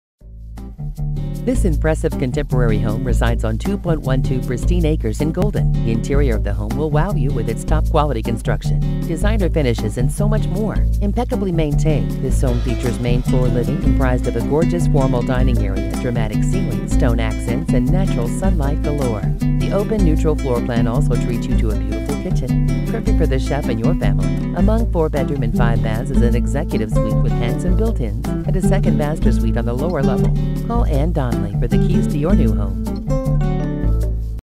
1870 Larkspur Drive Golden, CO Homes for Sale | coloradohomes.com
1870 Larkspur Drive, Golden, CO

Impressive contemporary with main floor living on 2.12 pristine acres. Executive suite with handsome built-ins. open, neutral floor plan. Top quality construction with designer finishes. 2nd master suite on lower level. 9' ceilings. Impeccably maintained.